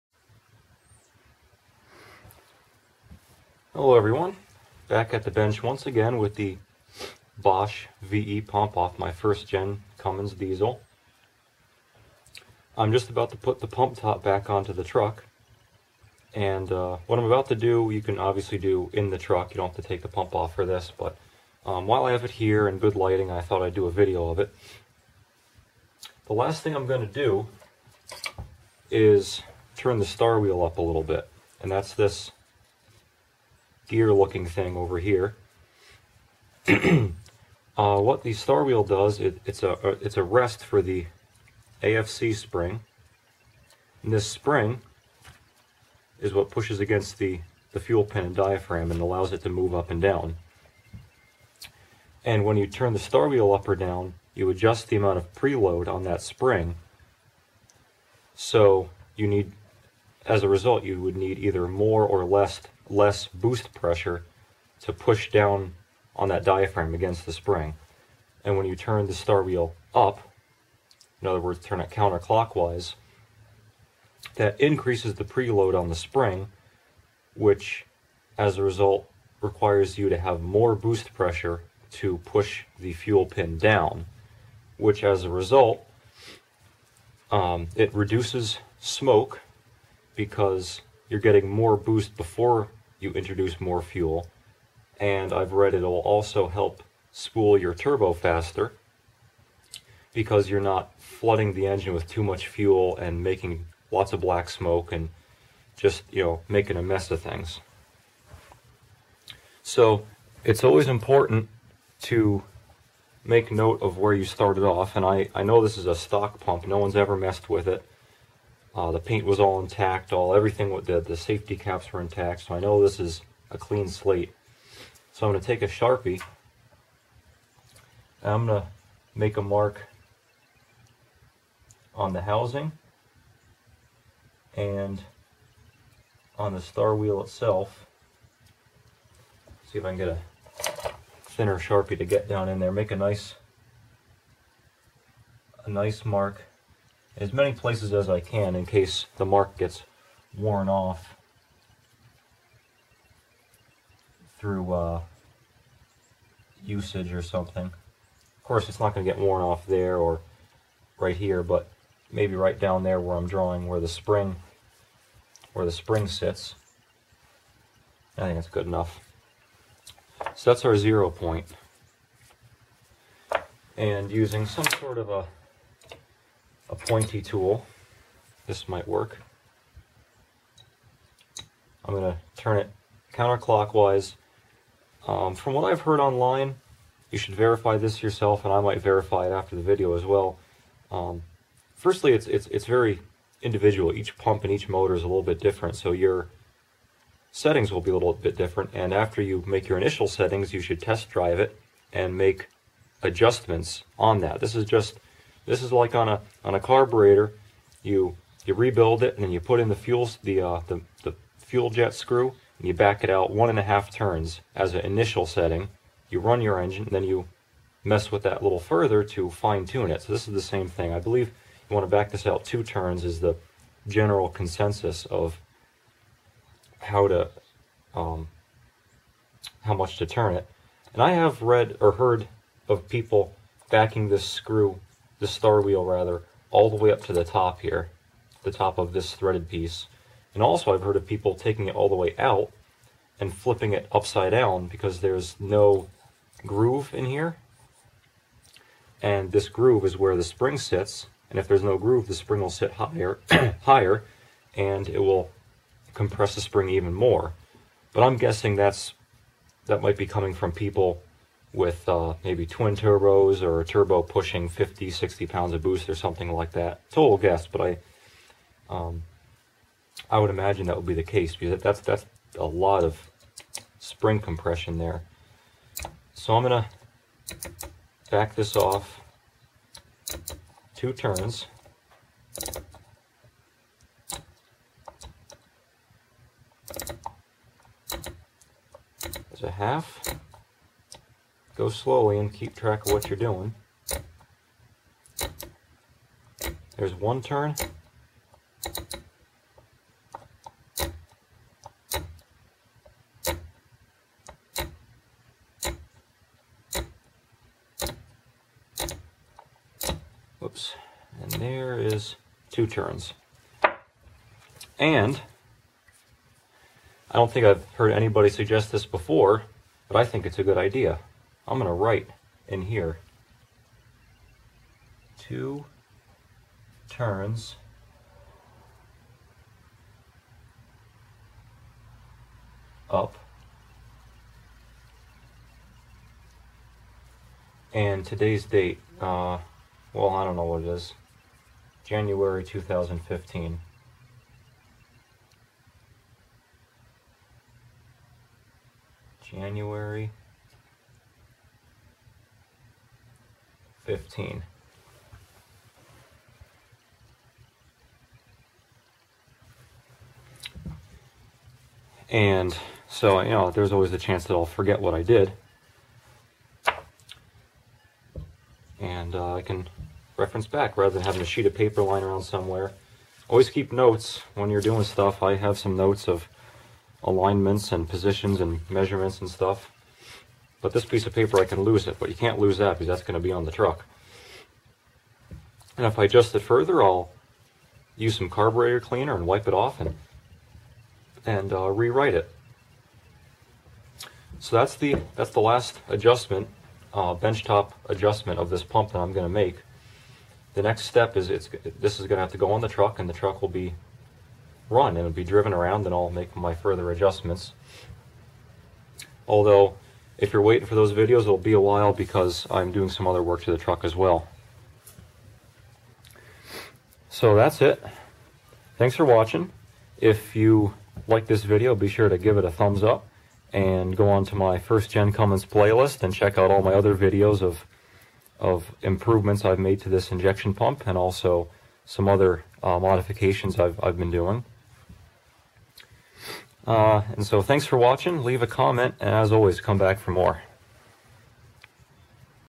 First Gen Dodge Cummins - Turning the Starwheel
The starwheel is an adjustment that allows you to change the AFC spring preload so the fuel pin advances down it's bore under more or less boost. This allows you to take care of low-end smoke and improve turbo spooling caused by excessive fueling in the low or no boost condition.